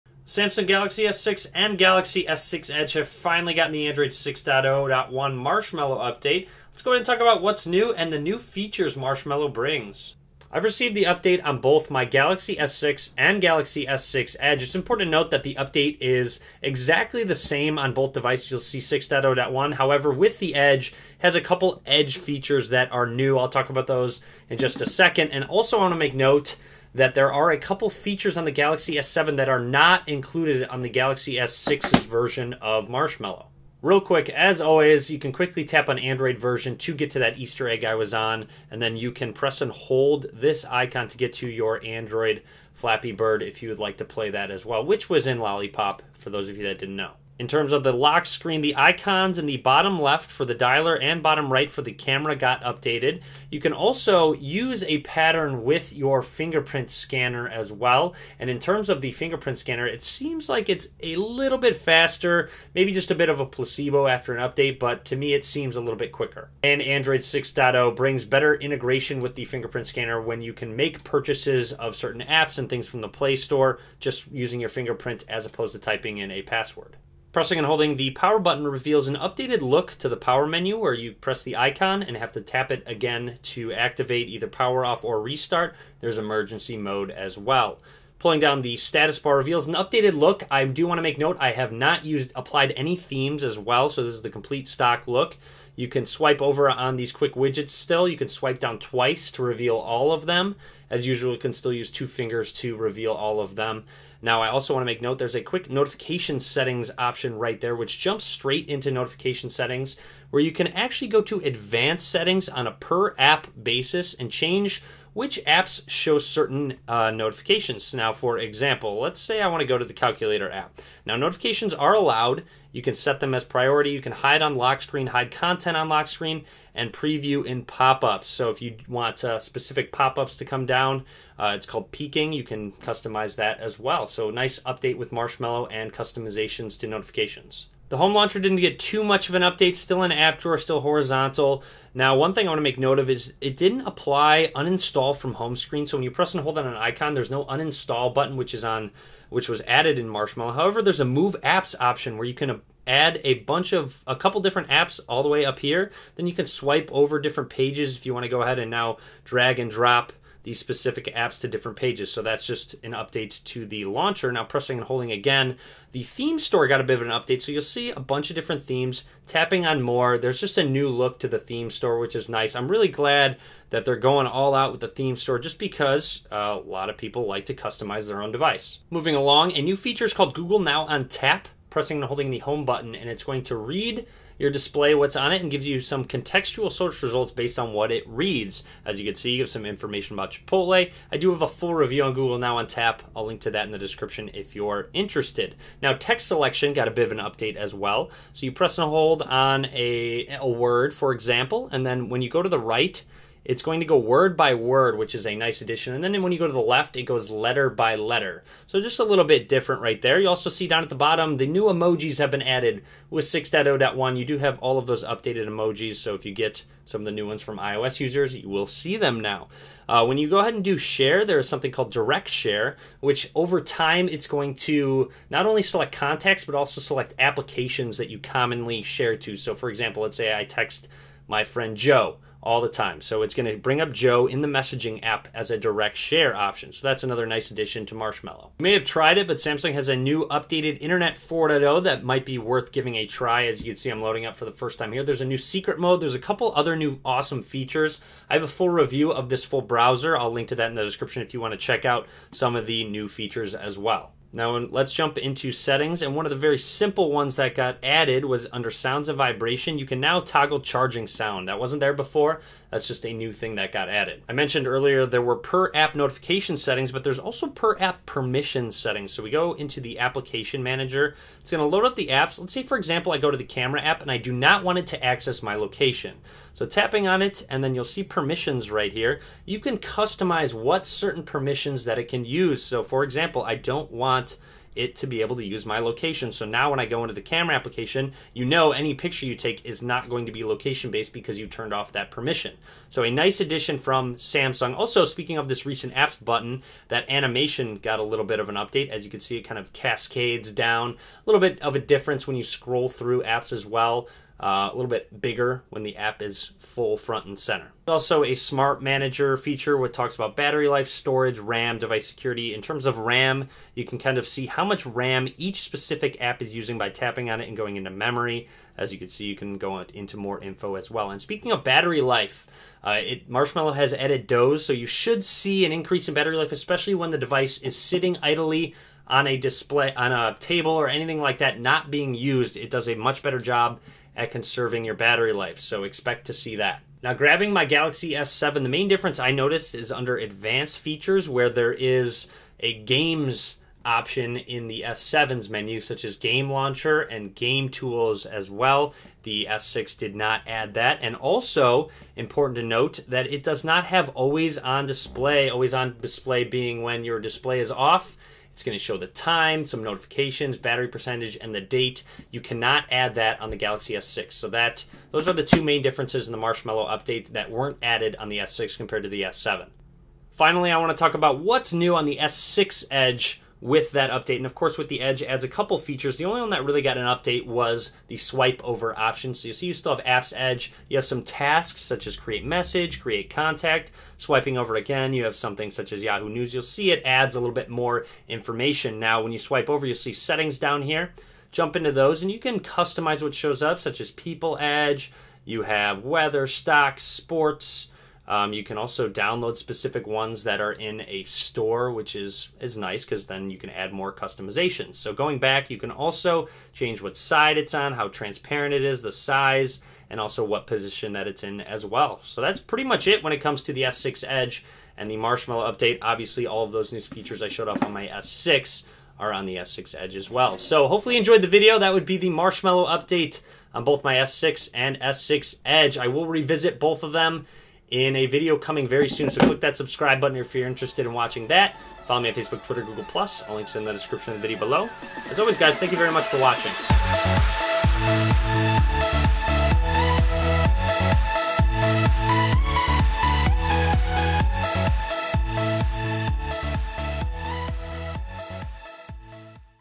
Galaxy S6 (Edge) Android 6.0.1 Marshmallow Update
Samsung is pushing out the Android 6.0.1 Marshmallow Update to the Galaxy S6 and Galaxy S6 Edge. Here is everything that is new and different on the 2 devices!